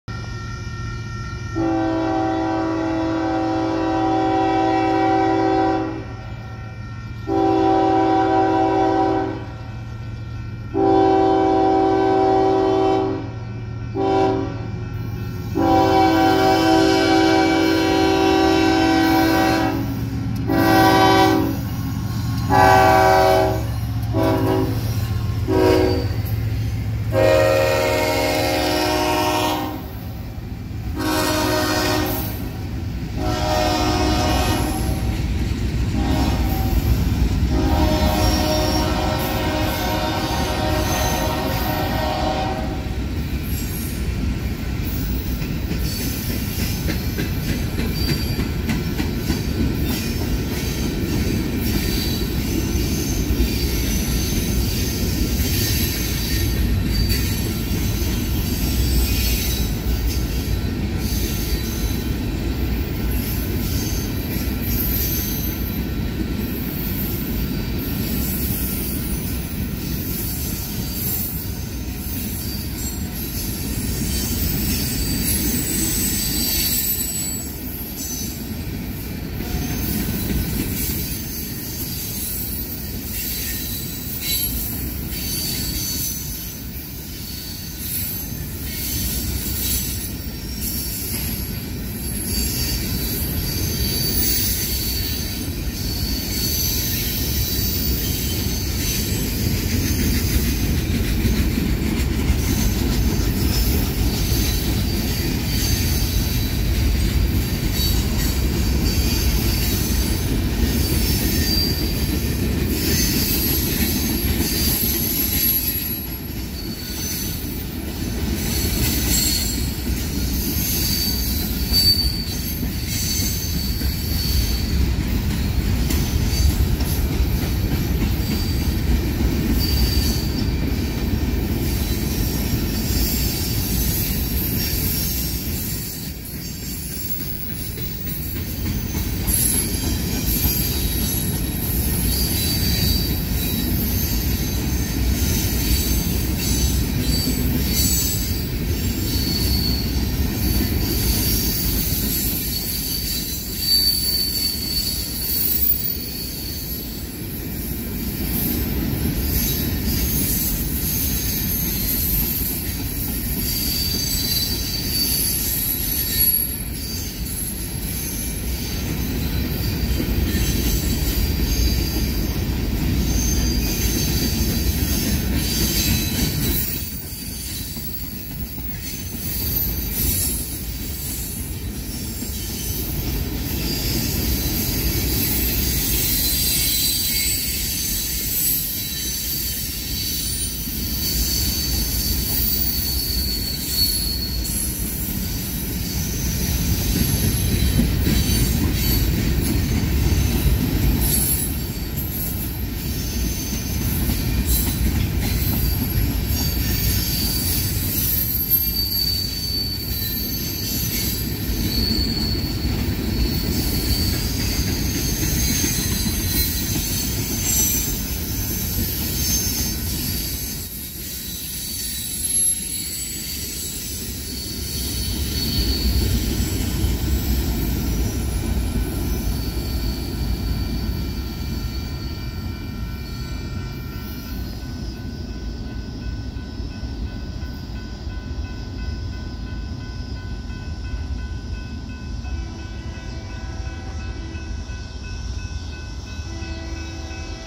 UP 7352 leads manifest train North through Navasota
Union Pacific 7352 leads this manifest train North through Navasota TX.